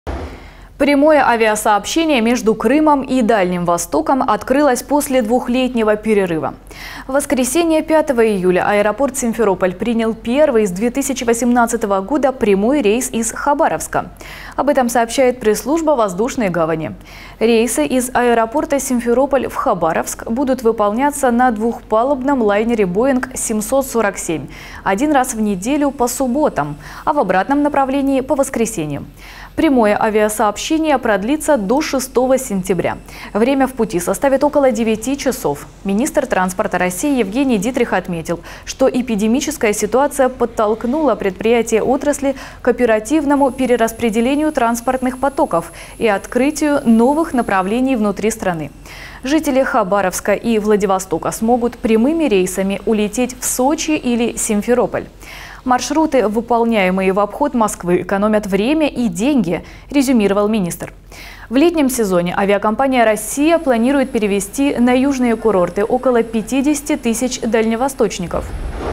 После двух лет перерыва самолеты полетели из Крыма на Дальний Восток
Прямое авиасообщение между Крымом и Дальним Востоком открылось после двухлетнего перерыва. В воскресенье 5 июля аэропорт «Симферополь» принял первый с 2018 года прямой рейс из Хабаровска. Об этом сообщает пресс-служба воздушной гавани.